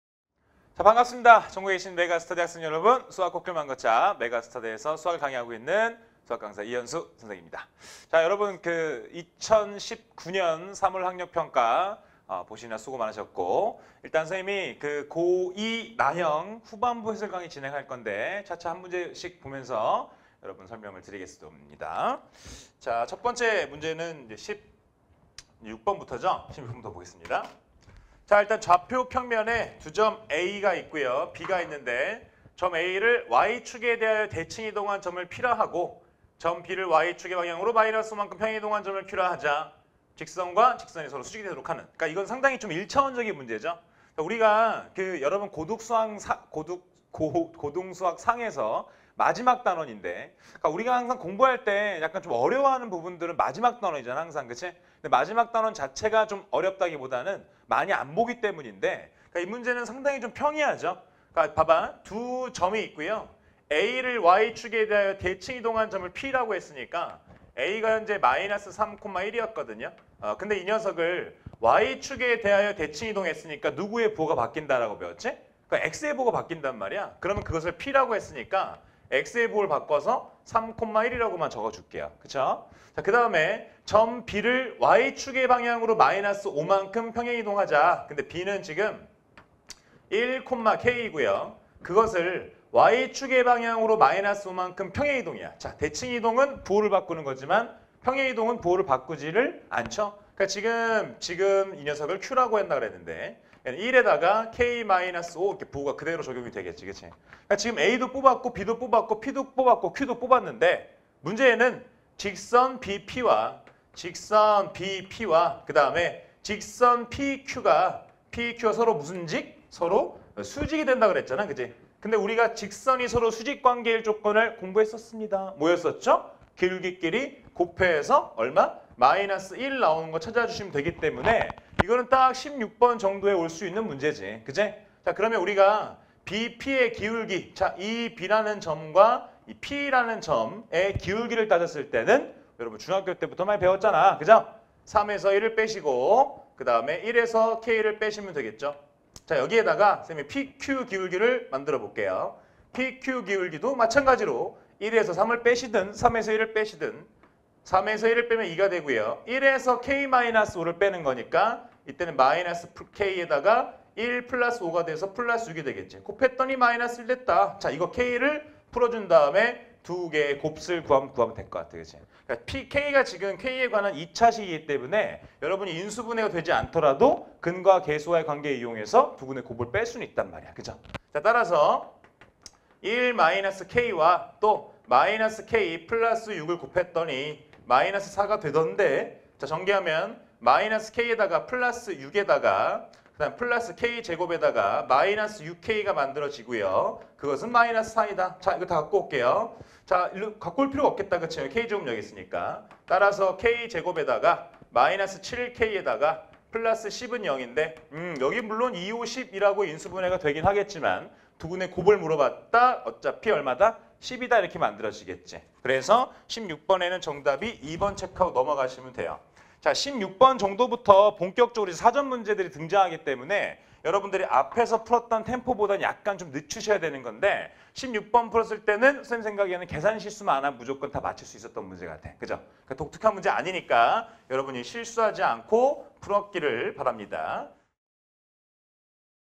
[메가스터디] 수학 이현수쌤 - 2019 3월 고2 학평★고2 수학(나) 해설강의★
2019 3월 고2 학평 수학(나) 해설강의
지금 이현수 선생님과 확인하세요

- 이현수 선생님의 강의가 더 궁금하다면?